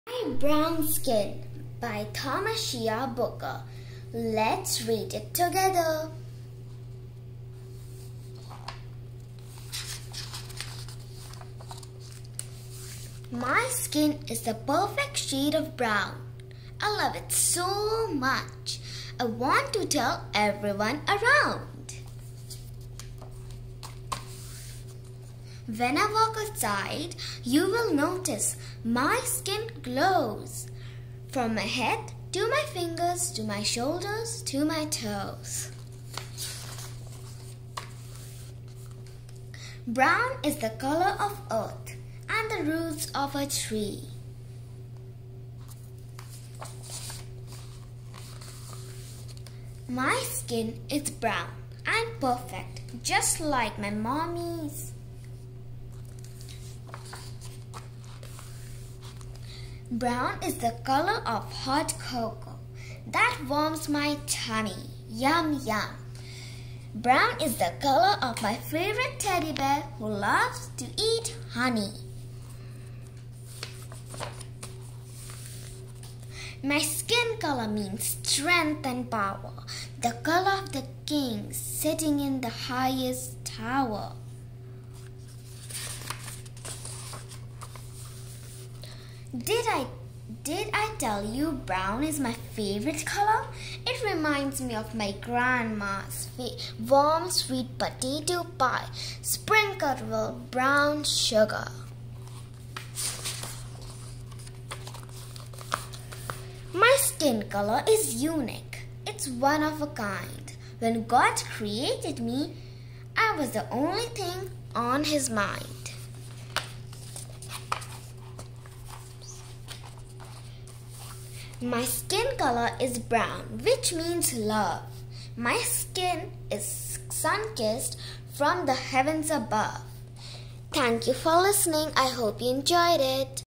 My Brown Skin
A heartwarming story about a brown girl full of confidence and pride to tell the world her skin is perfect shade of brown !